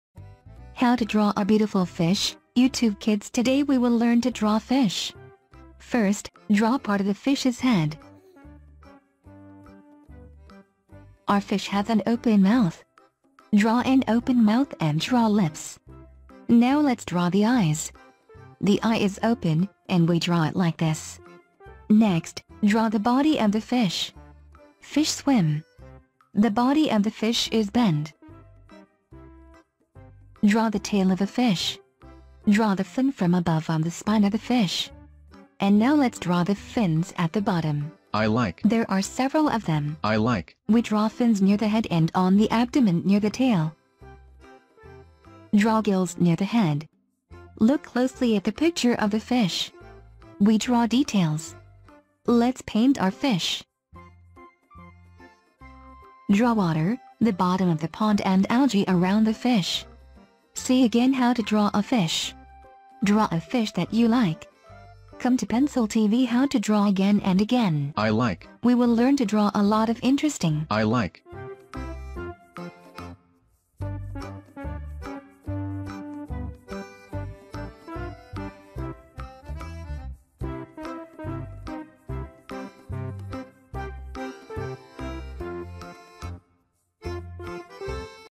How to draw a beautiful fish
Today we will learn to draw fish.
0:01 First, draw part of the fish's head.
0:05 Our fish has an open mouth. Draw an open mouth and draw lips.
0:14 Now let's draw the eyes. The eye is open, and we draw it like this.
0:21 Next, draw the body of the fish. Fish swim. The body of the fish is bent.
0:34 Draw the tail of a fish.
0:54 Draw the fin from above on the spine of the fish.
And now let's draw the fins at the bottom. There are several of them. We draw fins near the head and on the abdomen near the tail.
Draw gills near the head.
Look closely at the picture of the fish.
We draw details.
Let's paint our fish.
Draw water, the bottom of the pond and algae around the fish.
See again how to draw a fish. Draw a fish that you like.
Come to @PencilTVHowtodraw again and again. We will learn to draw a lot of interesting things!